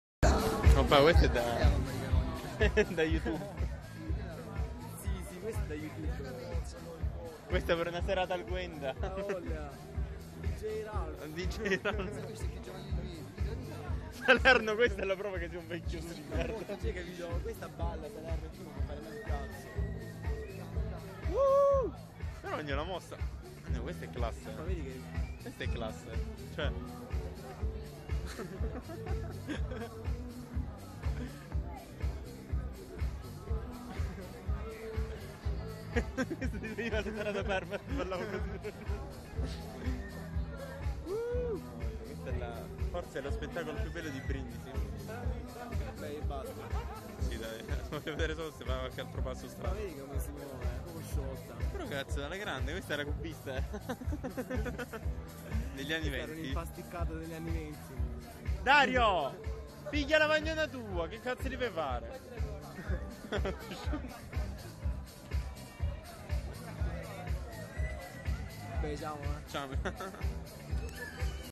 Vecchia Gwenda Ralf al Bar Betty di Brindisi
Guardate con quale estro e scioltezza si muove questa anziana signora sui ritmi della musica house...occhiali da sole.. ciabatte cn calze..drink in mano...merita di essere portata al Gwendalina.. Ammmmoooorrreeeeee :D